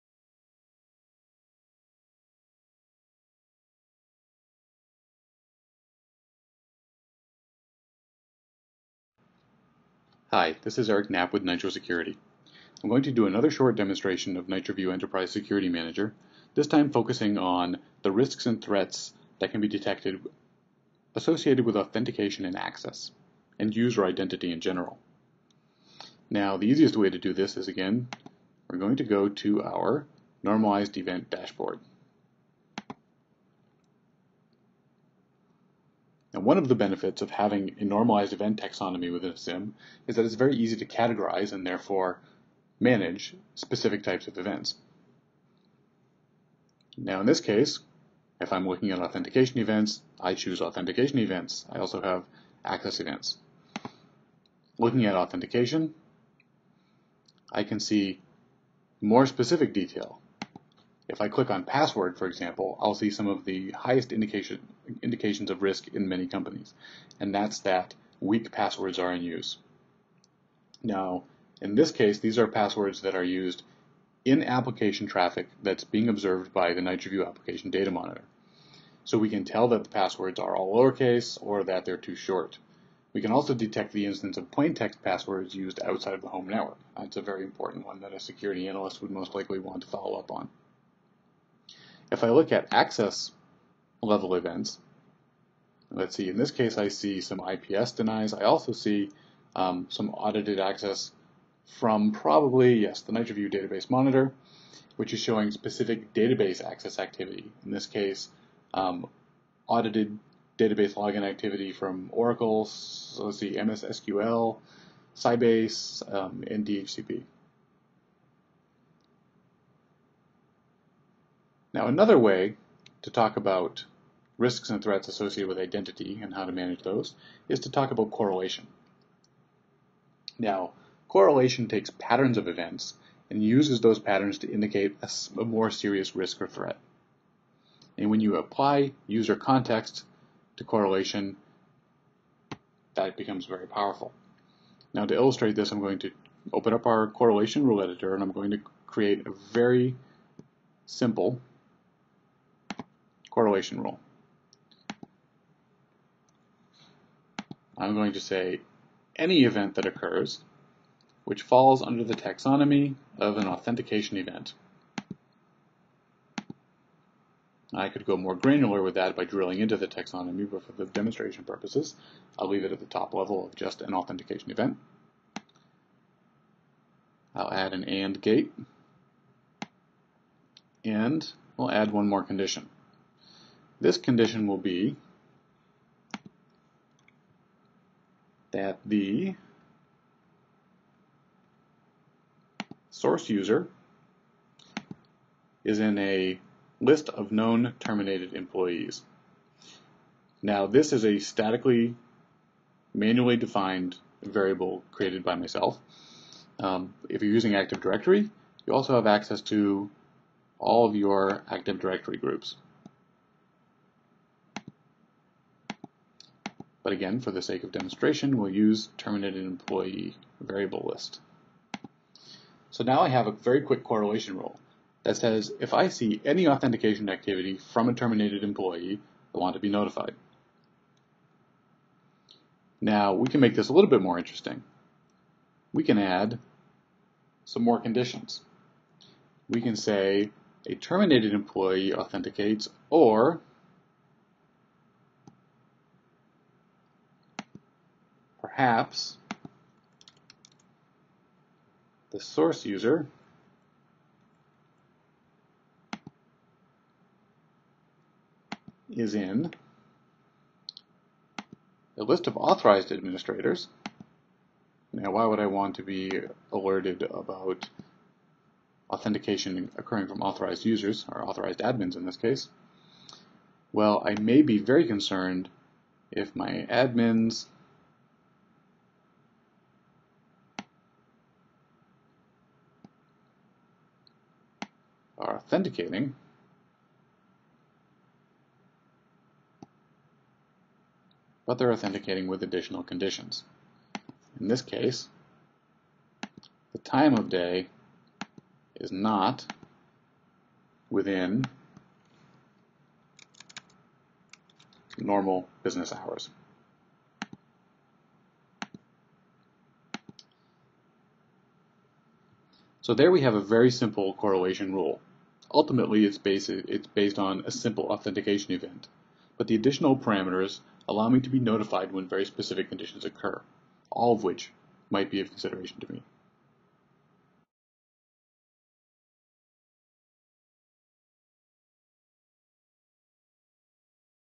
Users, roles, and authentication in event management
A short demo of NitroView ESM focusing on users, authentication and access.  Highlights how user and authentication context is useful within an overall security event taxonomy, and also how that context can be used to create meaningful event correlation rules.